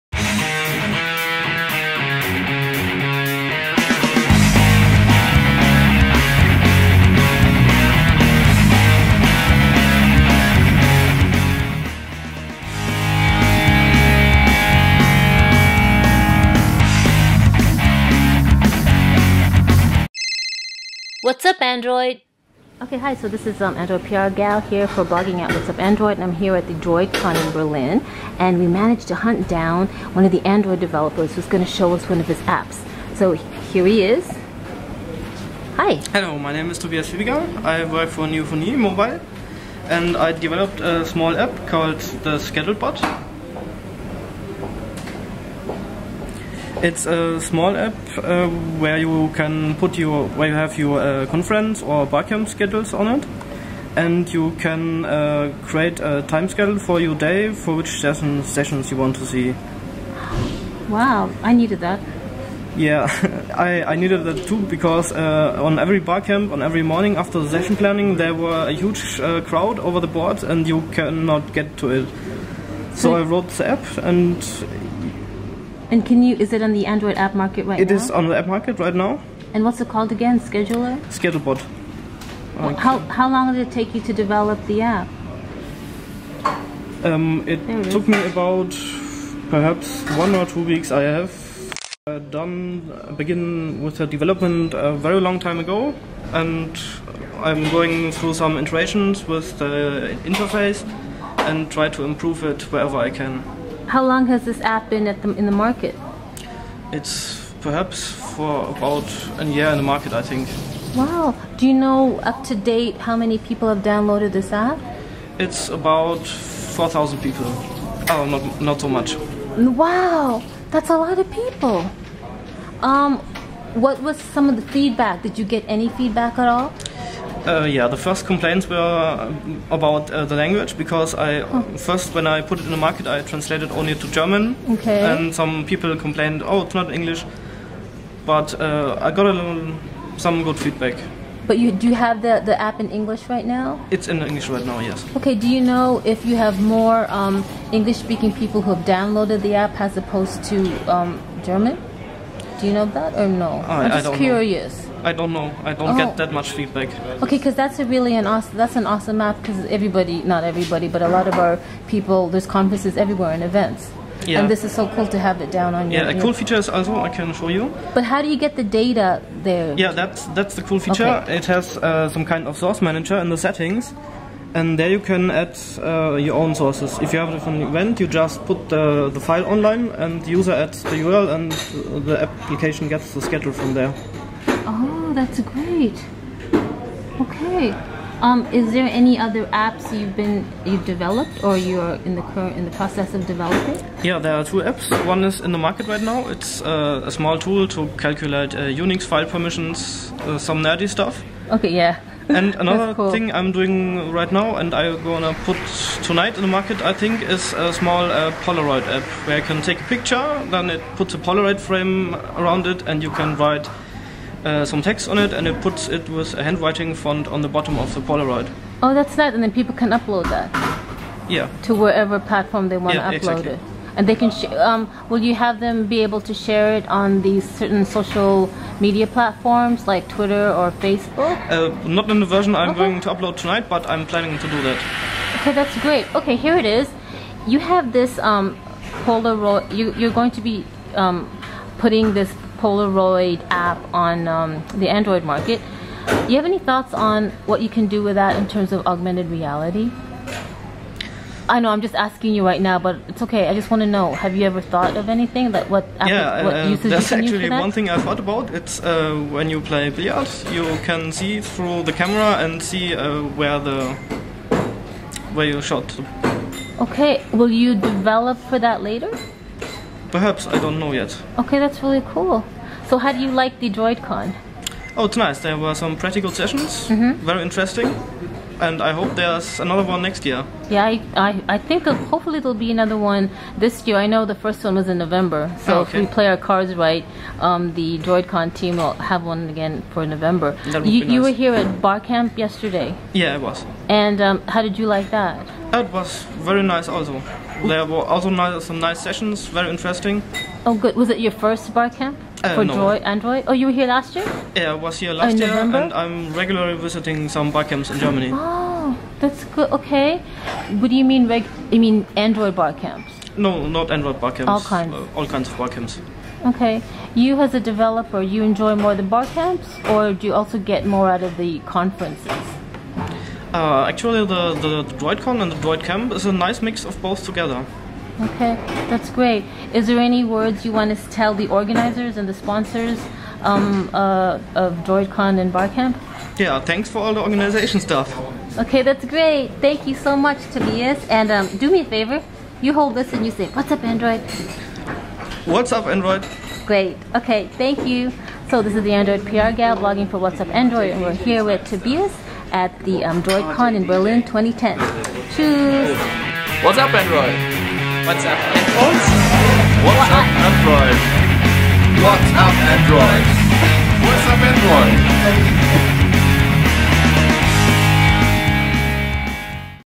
Schedule Bot Android App
Tobias Fiebiger, android developer who I met at DroidCamp/DroidCon in Berlin (May 26-27, 2010) showed us his Schedule Bot app that allows people to store conference schedules on their android phones. The app is available for download in the Android Market.